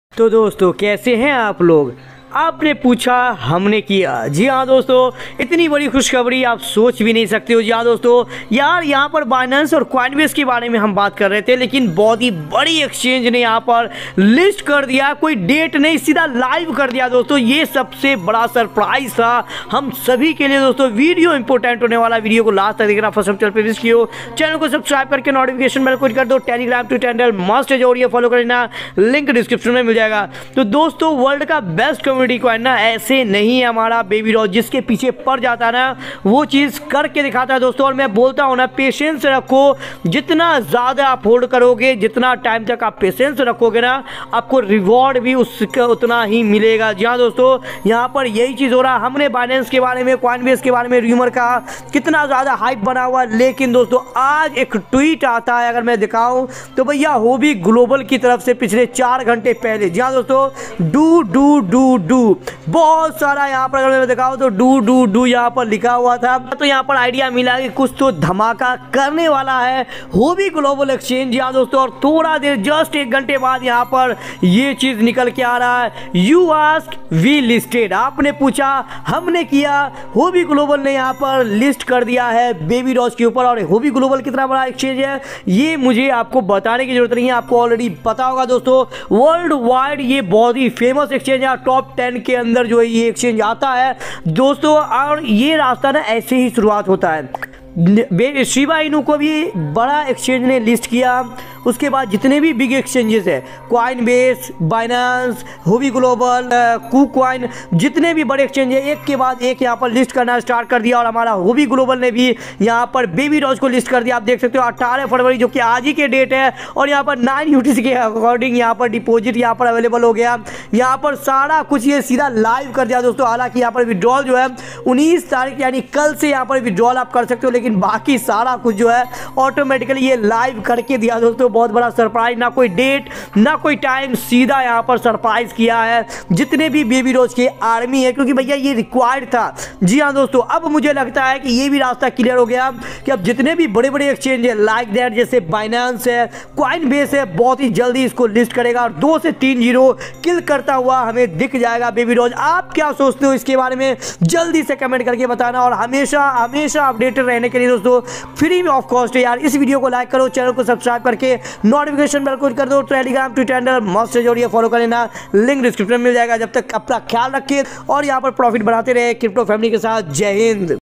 Baby Dogecoin List On Huobi Global🚀😱 | Reality Of Baby Dogecoin | Baby Dogecoin Army
To Dosto Aap Sabhi Ka Swagat Hai...
Aaj Is video me Main Meme Token Babydogecoin ke bare me Batane Jaa Raha Hu, Baby dogecoin kya hai, babydogecoin ka future kya hai, Baby dogecoin update and price prediction ke bare me bhi discuss kiya hai, Or Baby Dogecoin Aapko Kaise Millionaire Bana Sakta Hai...

Topic cover in this video :👇👇👇👇👇👇👇

1- what is babydoge coin in hindi
2- How to buy babydogecoin
3- best meme token to invest in
5- Reality of babydogecoin in hindi
6- Baby dogecoin latest update
7- Baby dogecoin future price prediction
8- babydogecoin kaise kharide